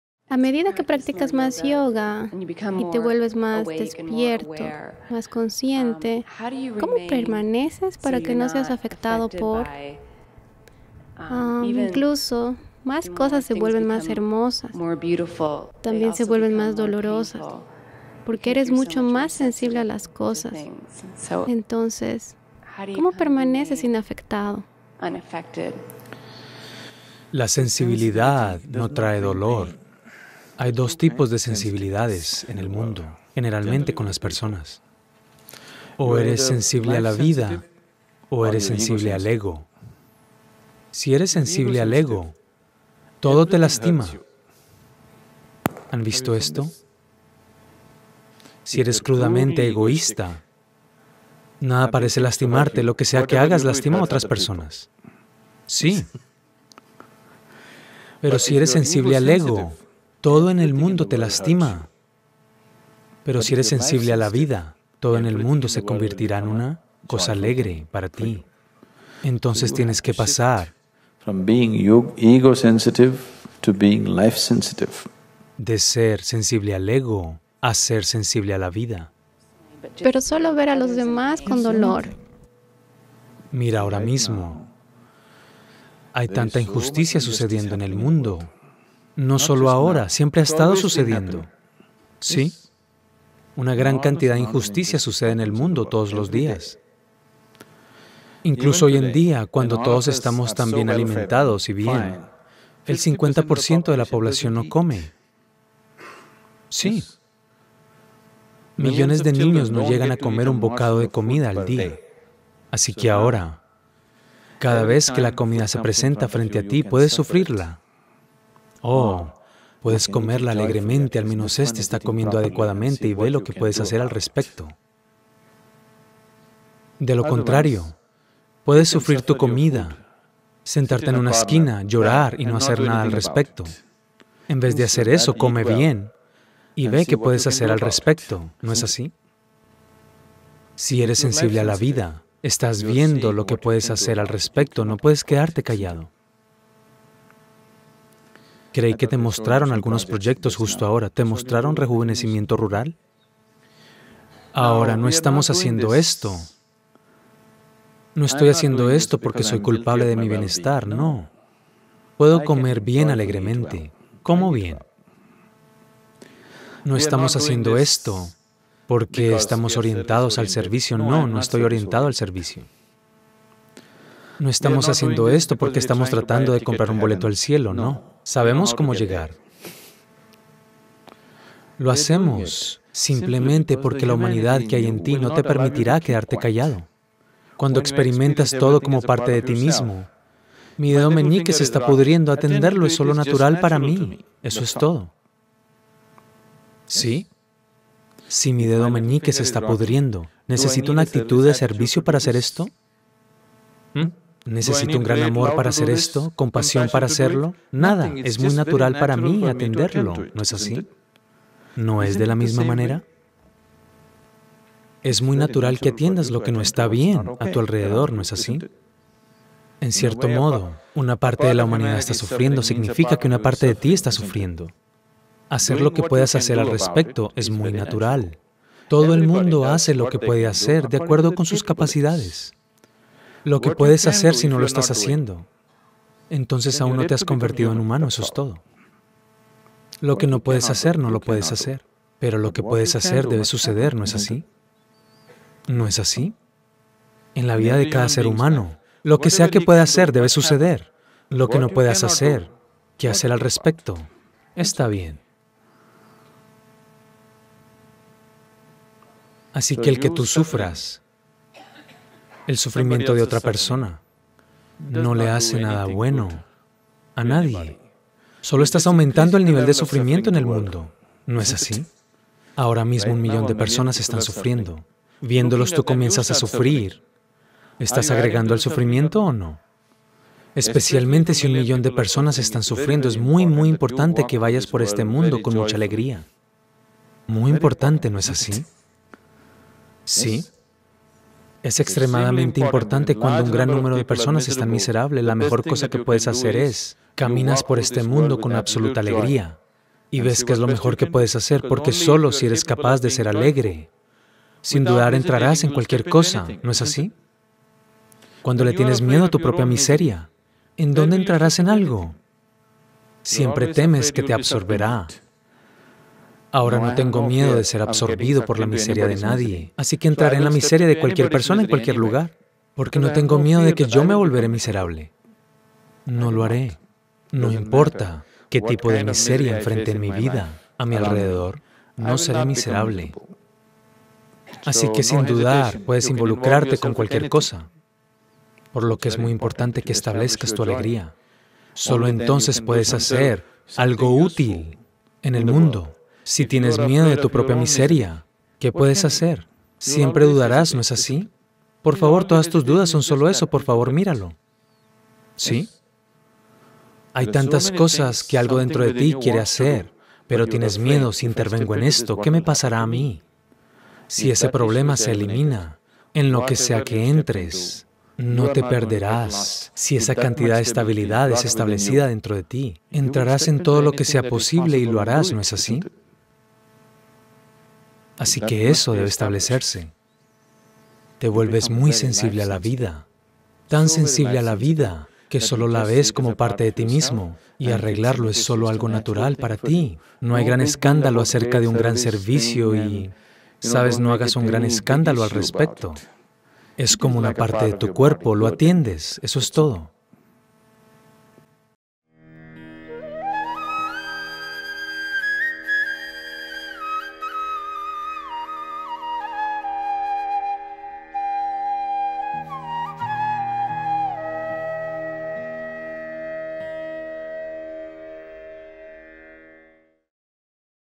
¿Eres sensible a la vida o eres ego-sensible?
¿Cómo reaccionar ante el sufrimiento ajeno?, ¿como permanecer centrado y ecuánime cuando nos enfrentamos cara a cara con la miseria que azota al mundo ?
Con suma compasión y claridad, Sadhguru responde la pregunta de una consteranda buscadora espiritual.